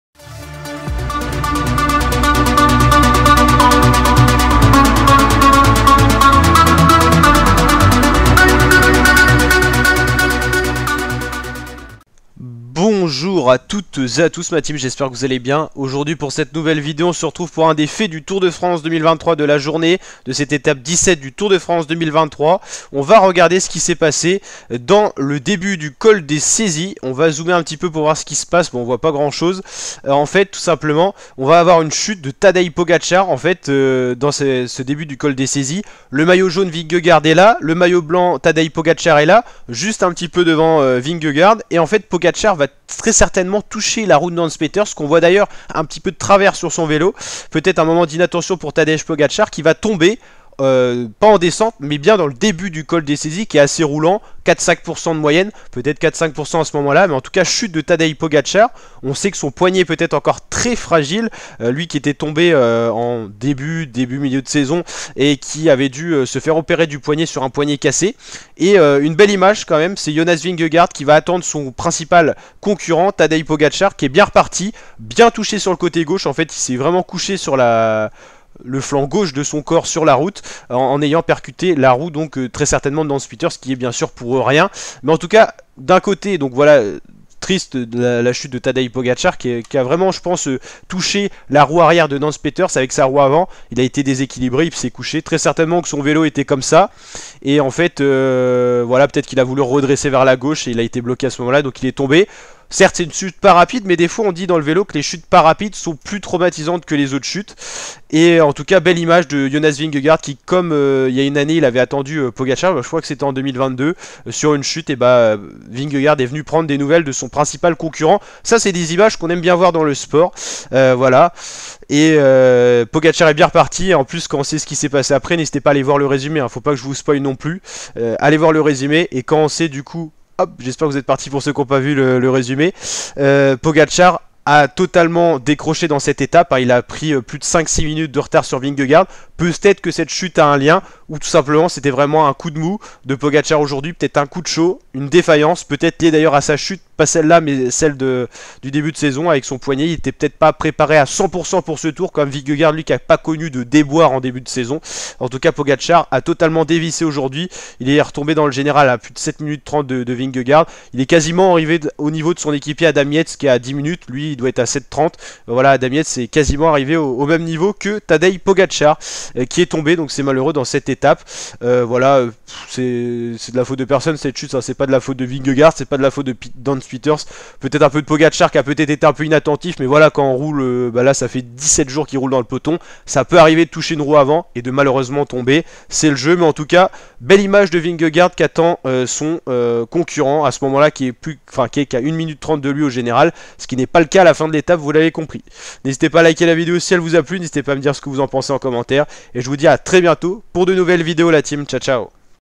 Chute de Tatej Pogacar Tour de France 2023 - étape 17 // Cyclisme
Chute de Tatej Pogacar Tour de France 2023 - étape 16 // Cyclisme